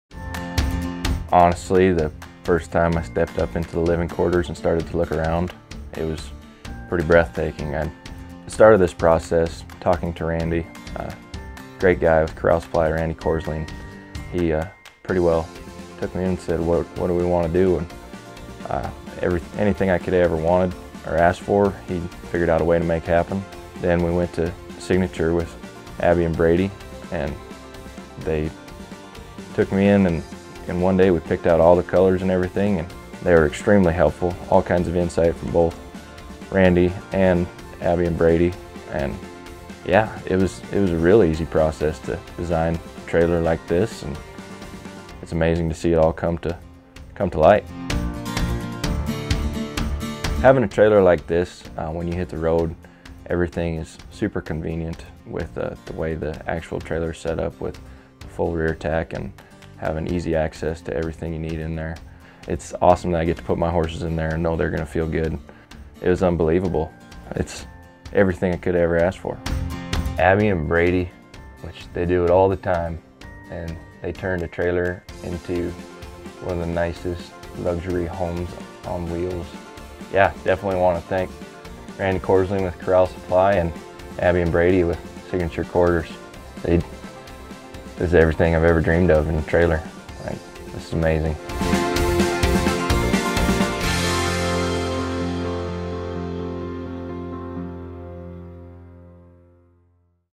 PRCA Steer Wrestler Bridger Anderson Signature Quarters Trailer Reveal
PRCA steer wrestler Bridger Anderson shows off his brand new Signature Quarters custom trailer interior and talks about his experience creating a new home on wheels.